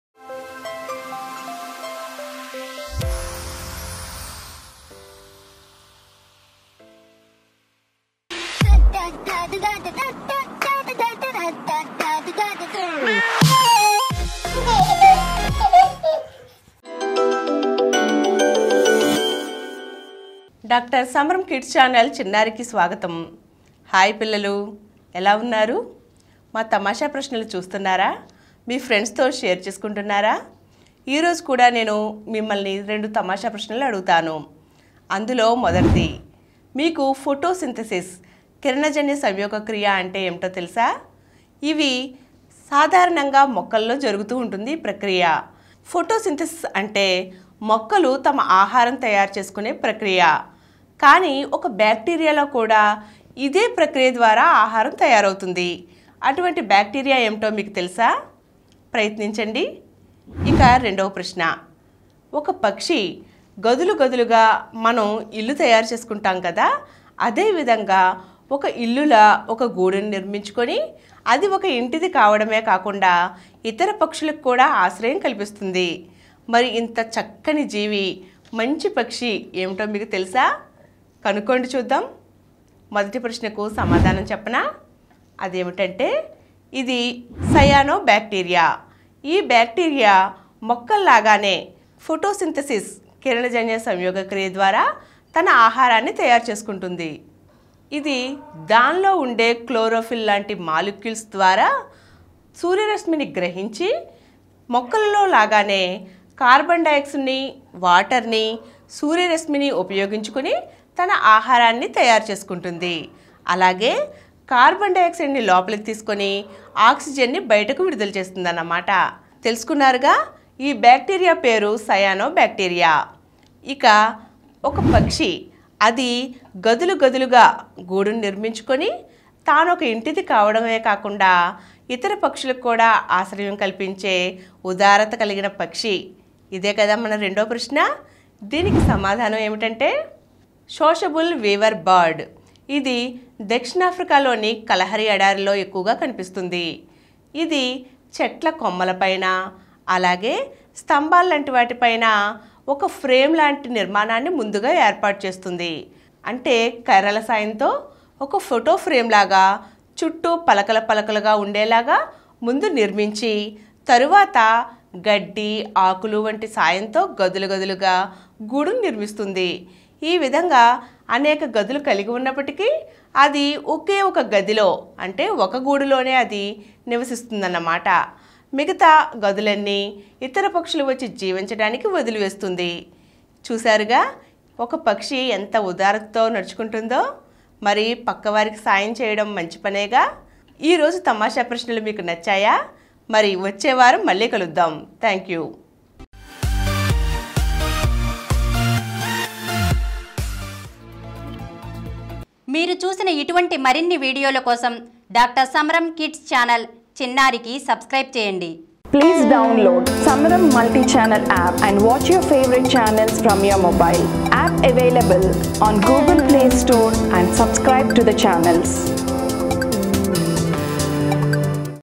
మొక్కల్లో లాగానే ఆహారాన్ని తయారు చేసుకునే బాక్టీరియా ఏమిటి ? (Epi -25) of కిడ్స్ బ్రెయిన్ టీజర్స్
(Episode-25) of Kids Brain Teasers Read by D.Lakshmi Padmaja in Dr Samaram Kids Channel CHINNARI